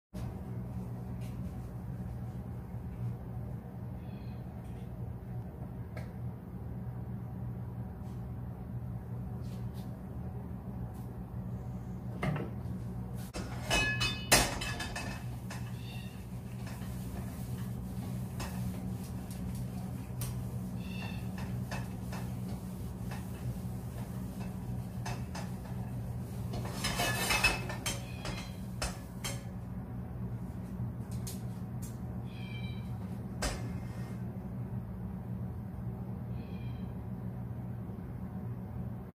Quick addition to seasoning on my Solidteknics pan
Needed to top up the seasoning on my Solidteknics pan so did a quick and dirty on the stovetop after washing it.